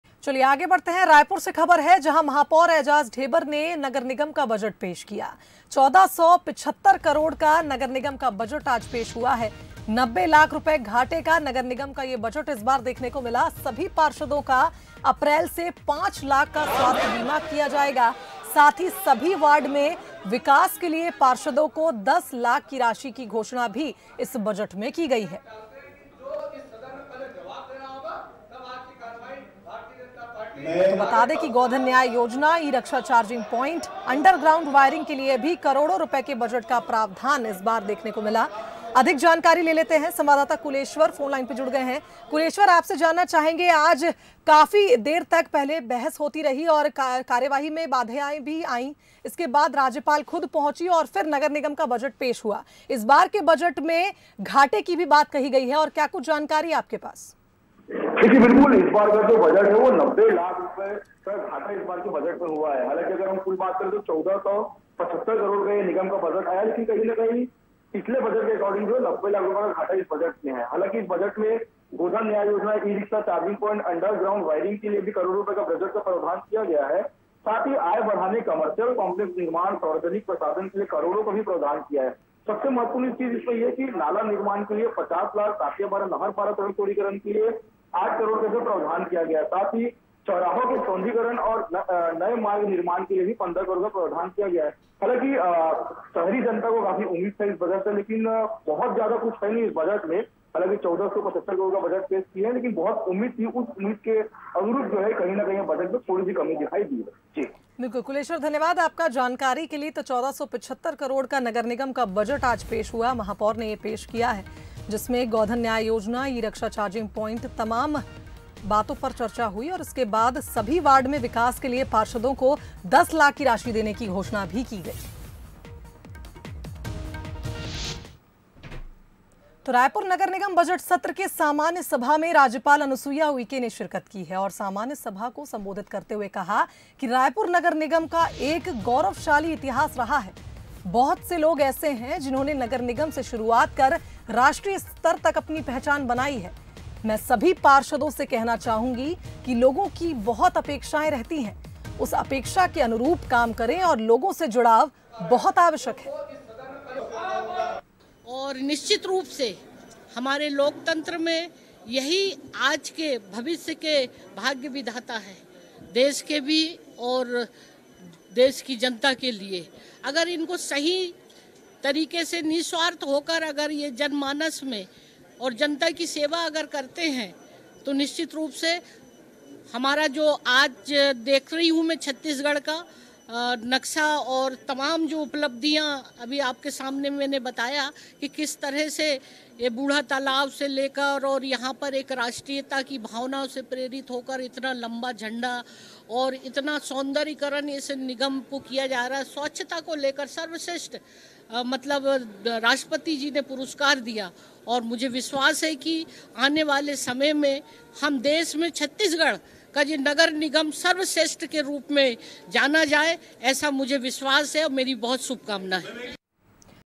महापौर ऐजाज ढेबर ने पेश किया नगर निगम का बजट,14 सौ 75 करोड़ का है बजट
90 लाख रुपए घाटे का है नगर निगम का बजटसभी पार्षदों का अप्रैल से 5 लाख का स्वास्थ बीमा करने की घोषणासभी वार्ड में विकास के लिए पार्षदों 10 लाख की राशि की घोषणा